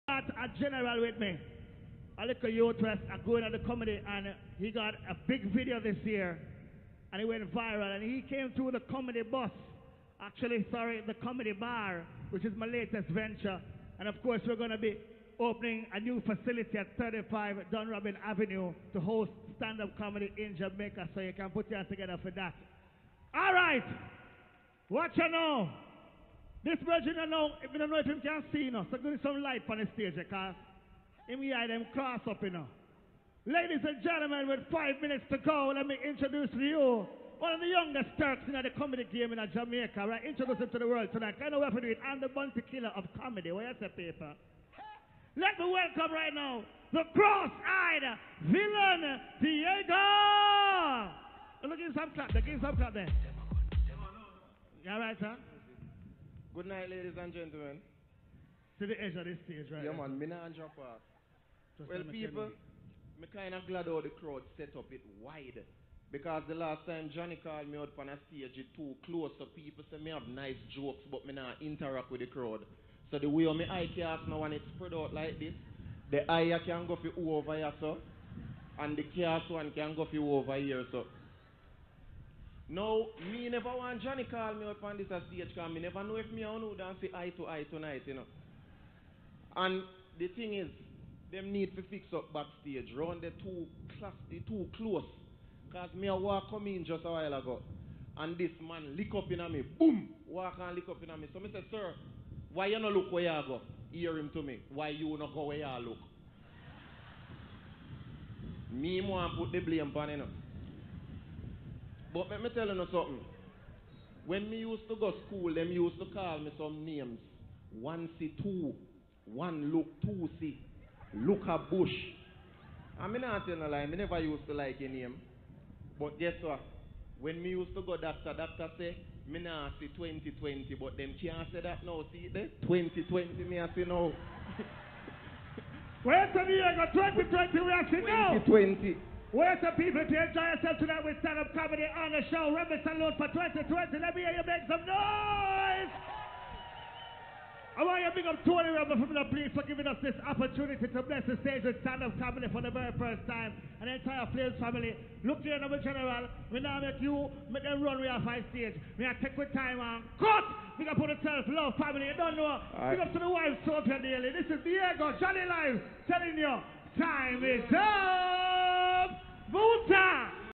Diego the cross eye villain at Rebel Salute 2020
Johnny the bounty killer of comedy introducing Diego to Rebel Salute 2020